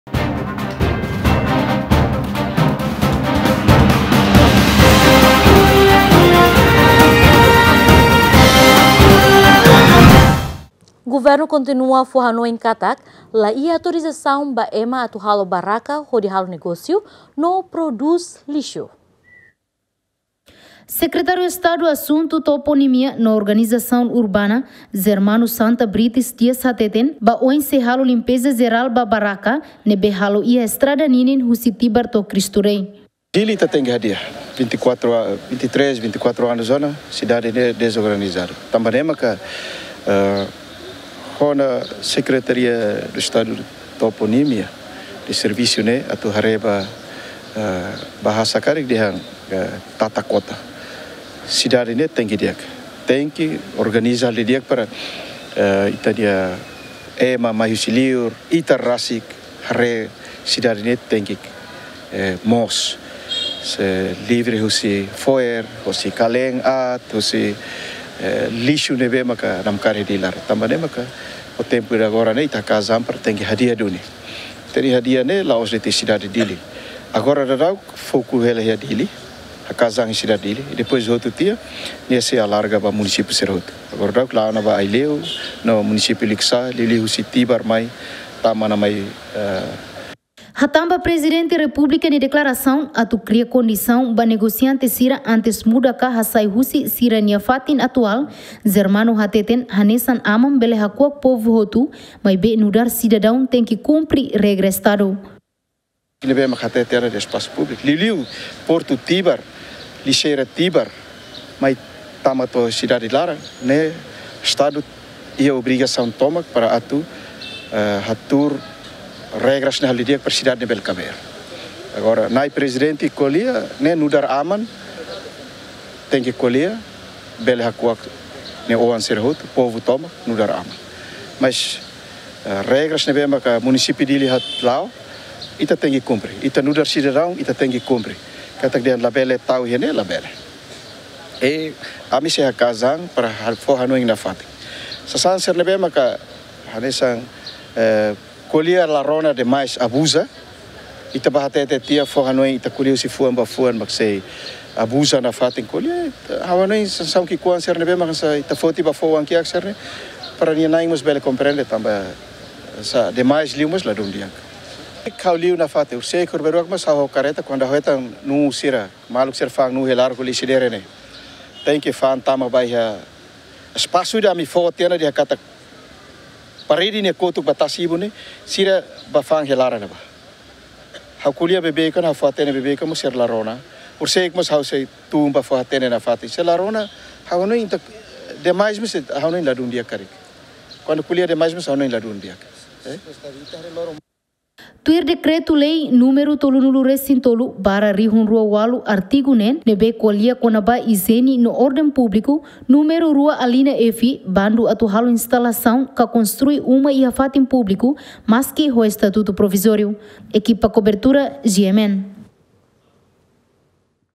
Governu kontinua fo hanoin laiha autorizasun ba ema atu halo baraka hodi halo negosiu no produs lixu
Governu kontinua fo hanoin katak laiha autorizasun ba ema atu halo baraka hodi halo negosiu no produs lixu